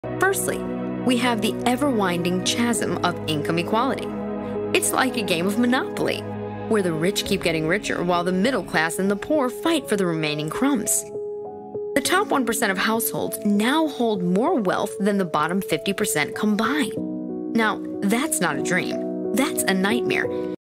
Income Equality: Is The American Dream Alive?
Is The American DREAM Alive? From the student debt crisis, the housing market crisis, income equality, to the abundant opportunities that James Truslow Adams envisioned for all Americans. Over the years, the American Dream has come under scrutiny, with some accusing it of being dead. In this video, I'm exploring the question: is the American Dream ALIVE? If you like this short check out the full video "Is The American Dream Alive" right here on the Culture Club USA YouTube channel!

Thanks for stopping by our new channel!